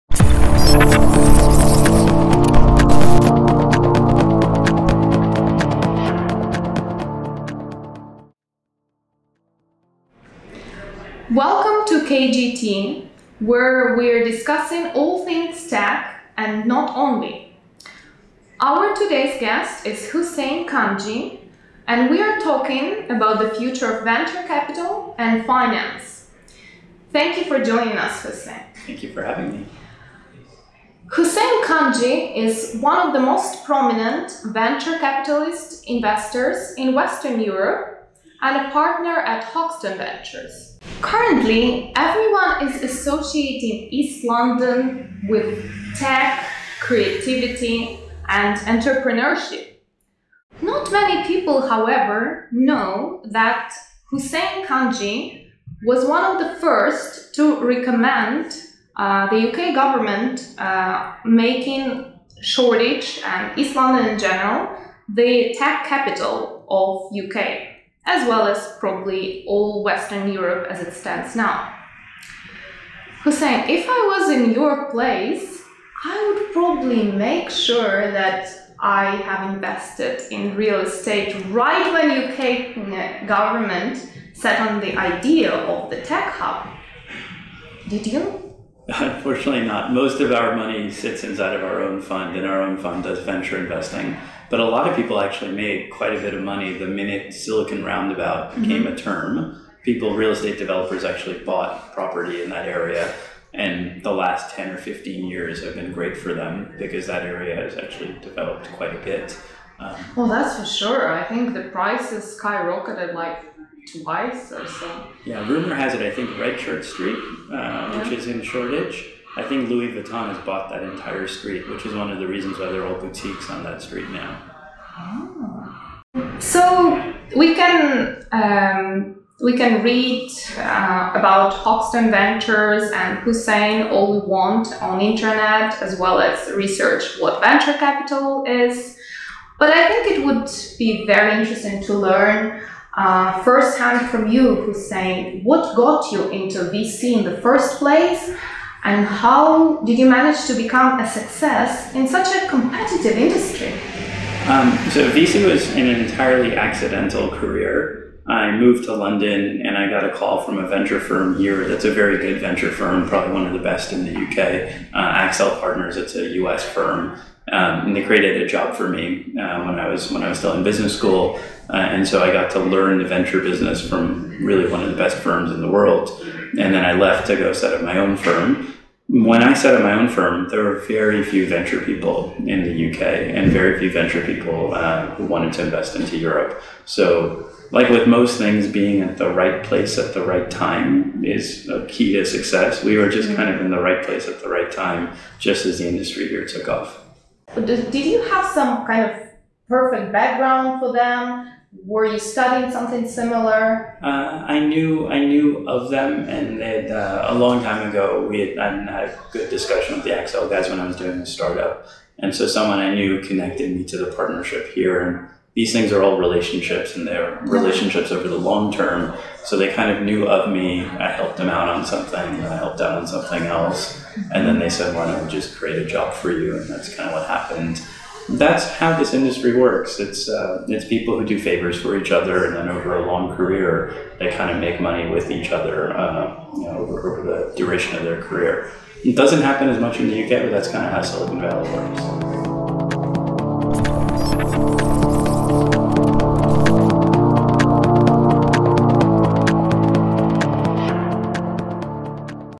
A Glimpse Into The World of Venture Capital
In this discussion with Hussein Kanji, Partner at Hoxton Ventures, we talk about the formation of East London as a Silicon Valley of West Europe and learn how to make a foot step into the exciting world of Venture Capital